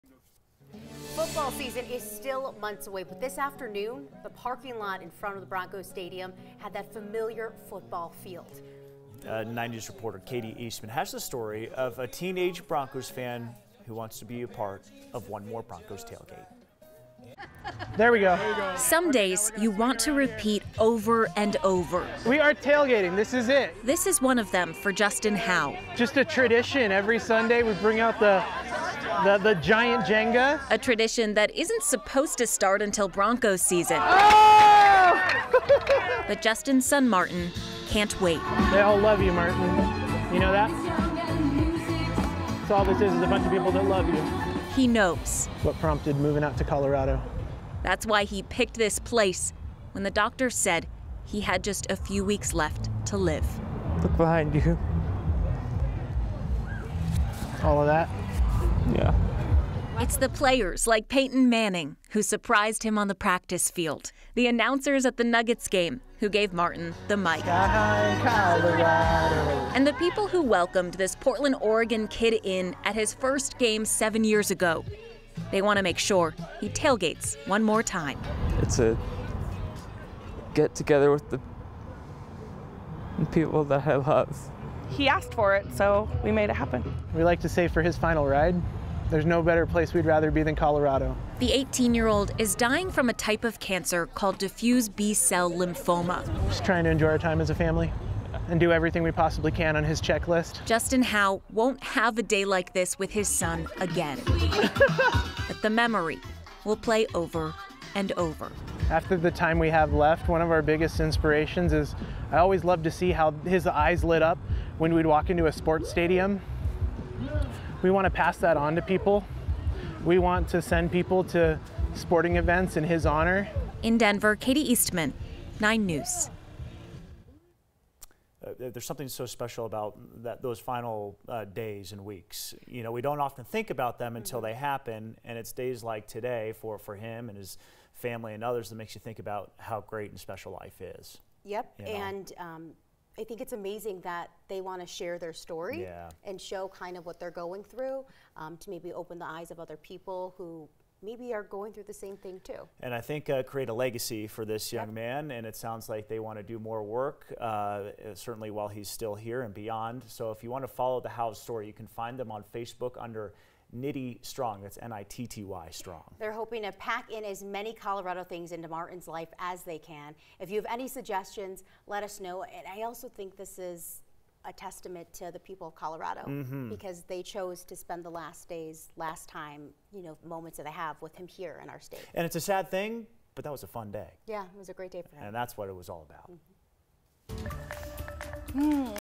A boy with only a few weeks to live is doing as many Colorado things as possible
A boy with only a few weeks to live got to experience a special Denver Broncos tailgate.

9NEWS (KUSA) is located in Denver, Colorado.